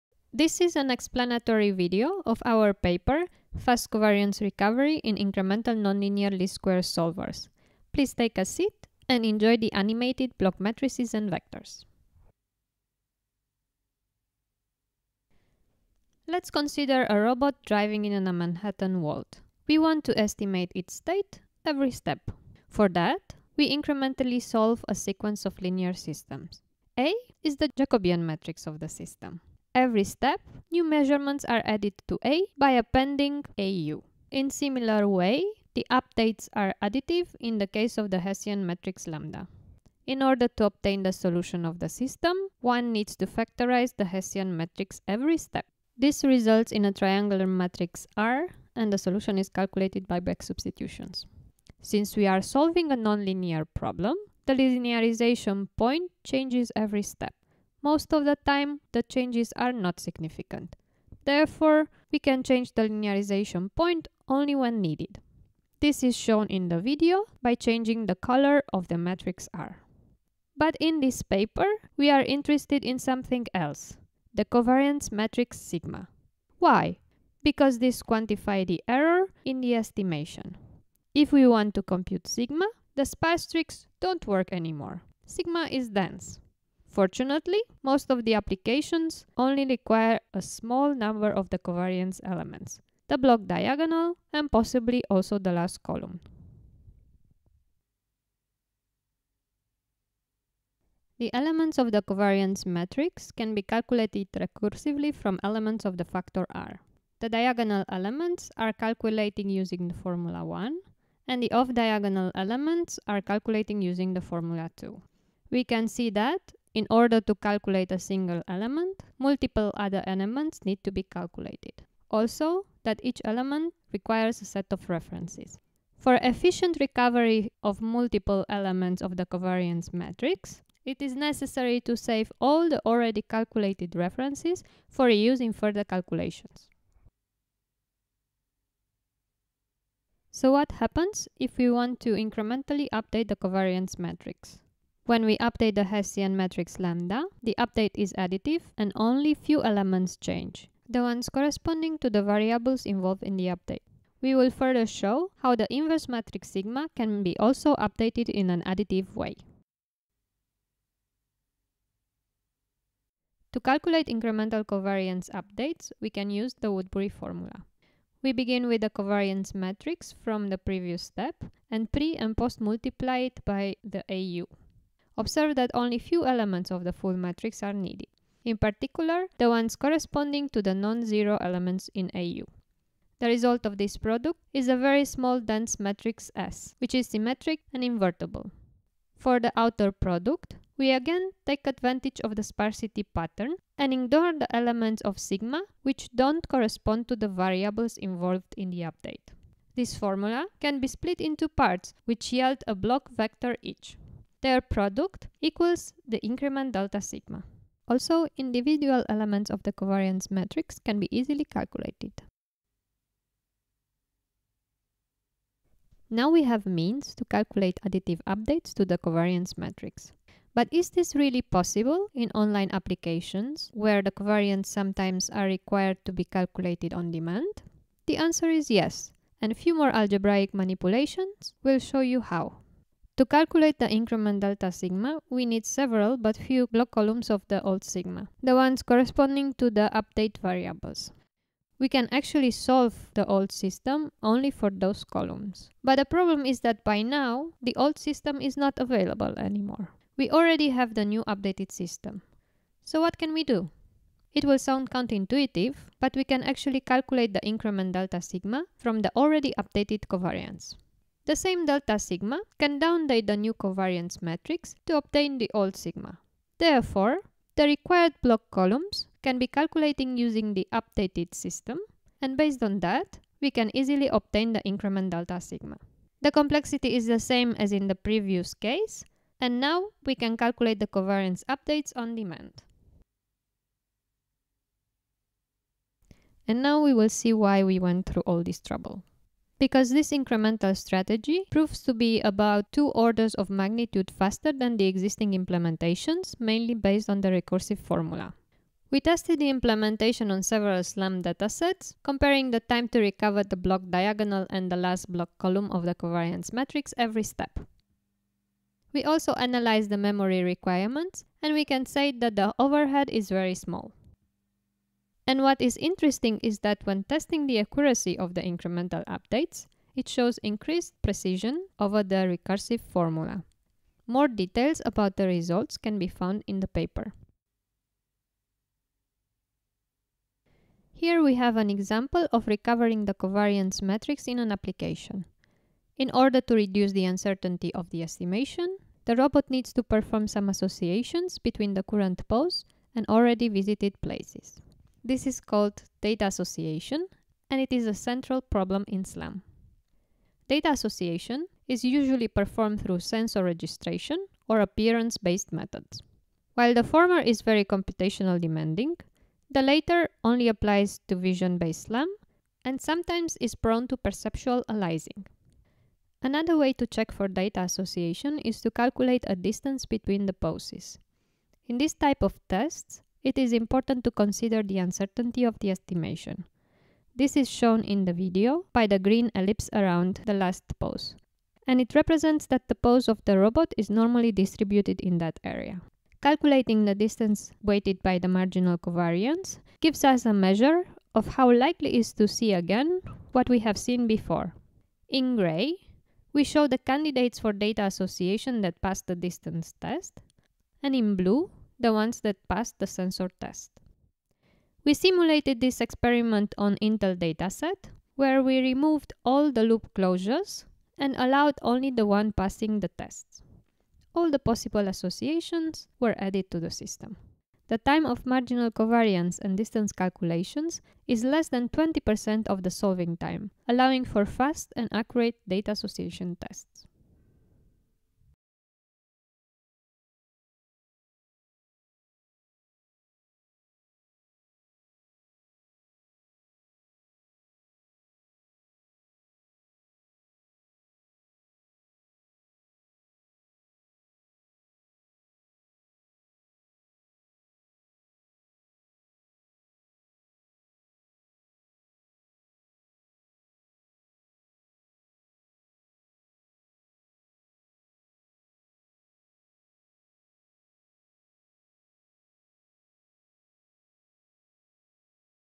SLAM++ | ICRA 2015 Video
Video for the paper "Fast Covariance Recovery in Incremental Nonlinear Least Square Solvers".